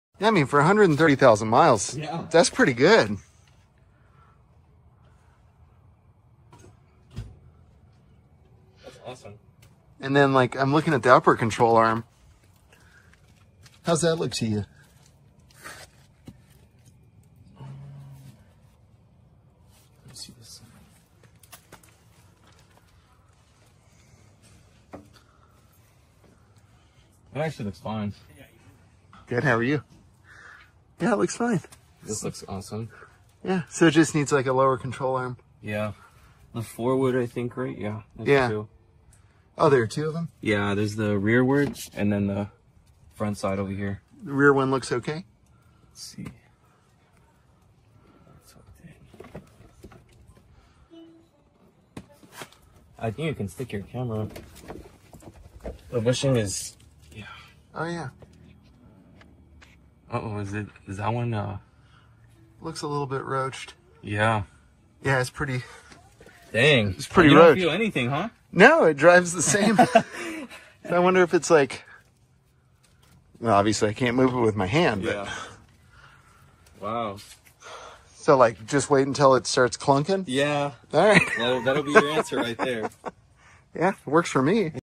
Control Arm Bushing: Wait for the CLUNK? 130k Miles Tesla Model 3
Checking the control arm bushings on a 2022 Tesla Model 3 with over 130K miles.